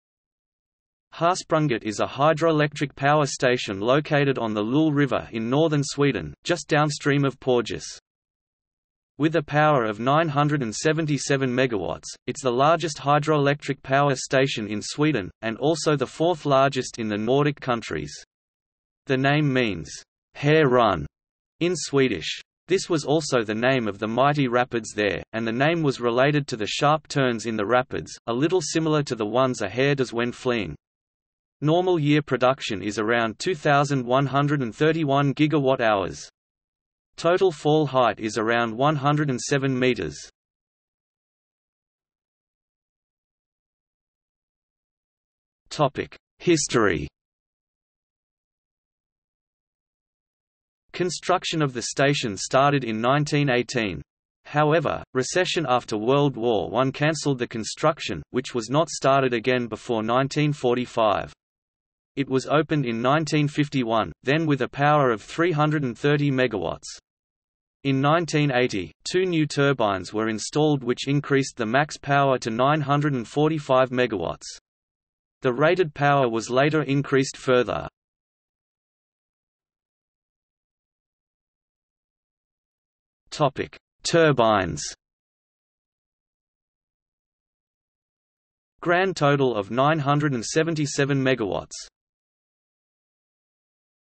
Harsprånget hydroelectric power station | Wikipedia audio article
00:00:41 History
00:01:19 Turbines

- Socrates

SUMMARY
Harsprånget is a hydroelectric power station located on the Lule River in northern Sweden, just downstream of Porjus.
With a power of 977 MW, it's the largest hydroelectric power station in Sweden, and also the fourth largest in the Nordic countries. The name means "Hare run" in Swedish. This was also the name of the mighty rapids there, and the name was related to the sharp turns in the rapids, a little similar to the ones a hare does when fleeing. Normal year production is around 2131 GWh. Total fall height is around 107 m.